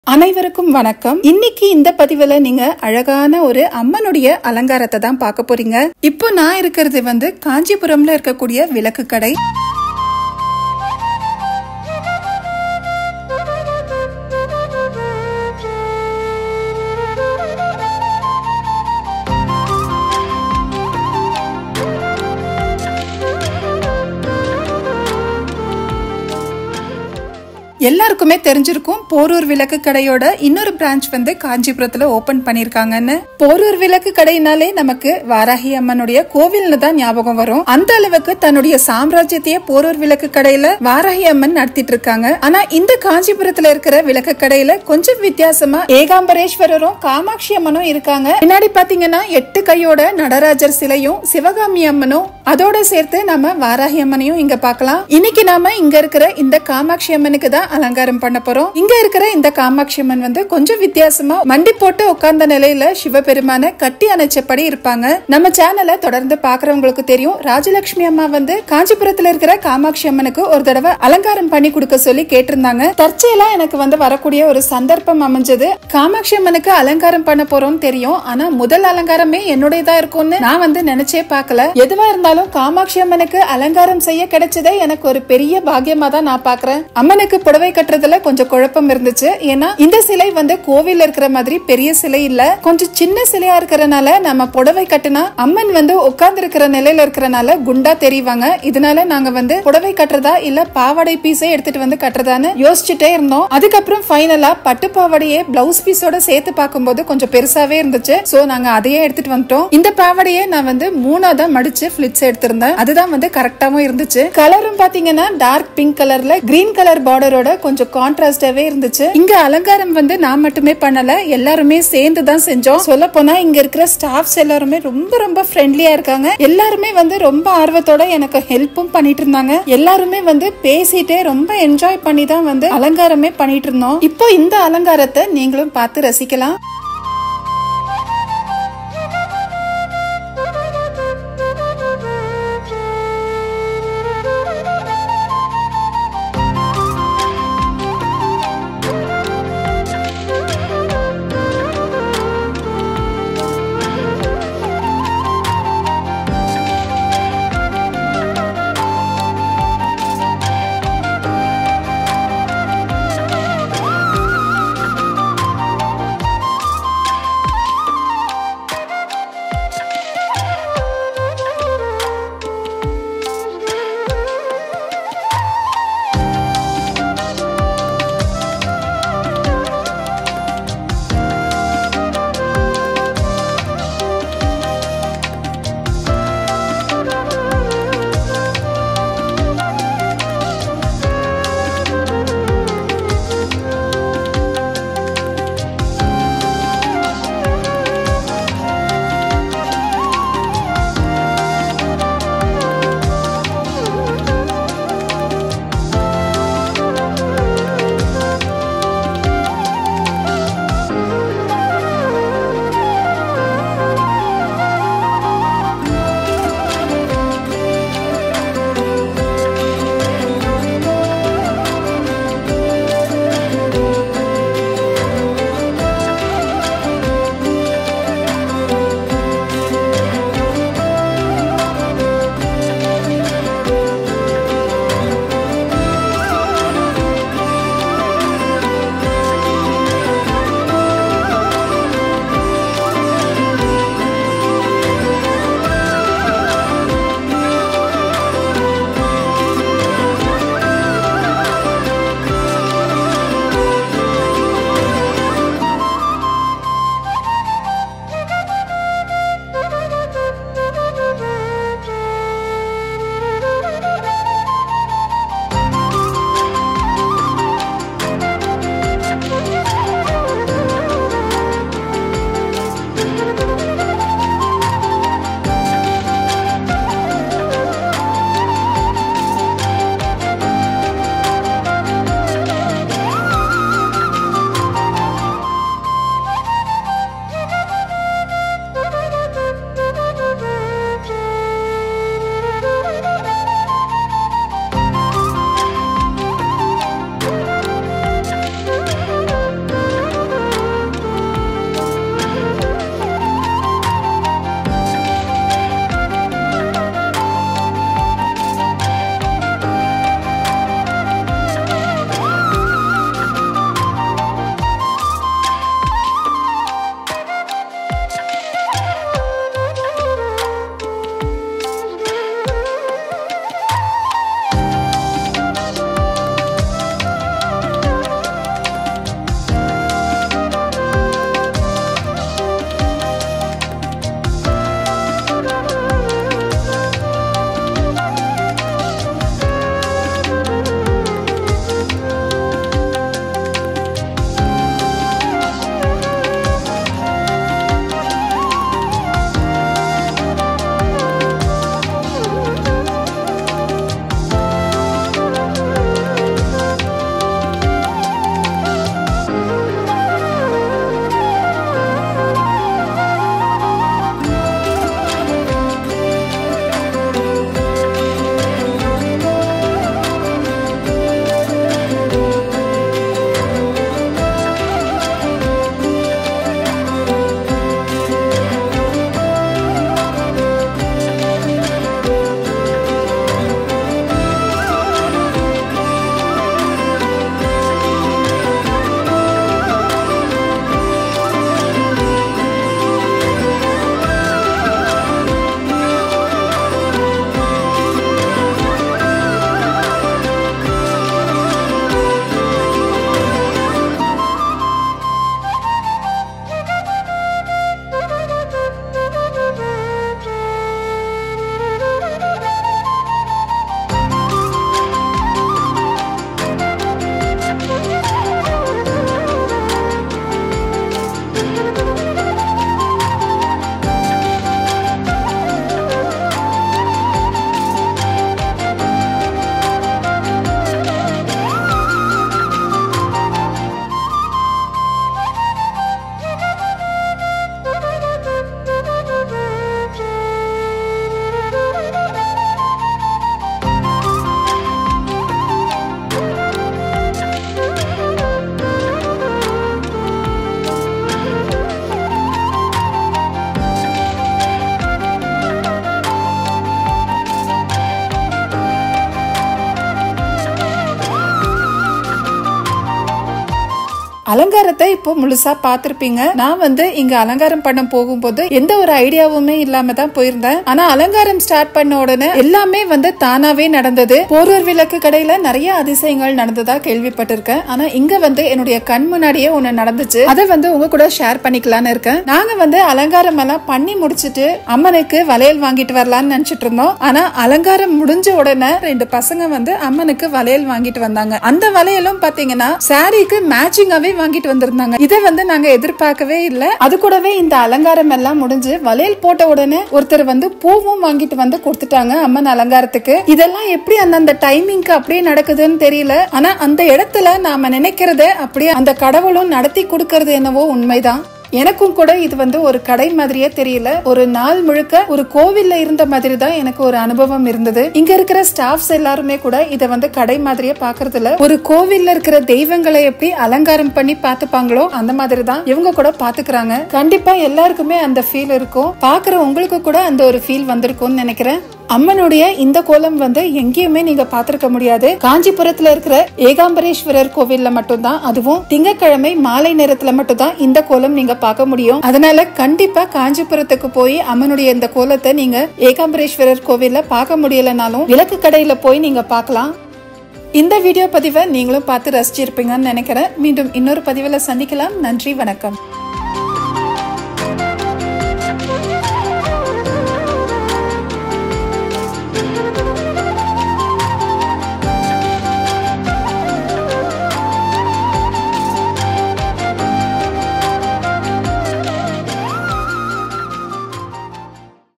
ஏகாம்பரேஸ்வரர் காமாக்ஷி அம்மன் அலங்காரம் || காஞ்சிபுரம் விளக்கு கடையில் ஒரு நாள் || TALE OF KANCHI
ADDRESS:-
RL Handicrafts Vilakkukadai,
30, Nadu Street, Sheikpet S St,
Kanchipuram, Tamil Nadu 631501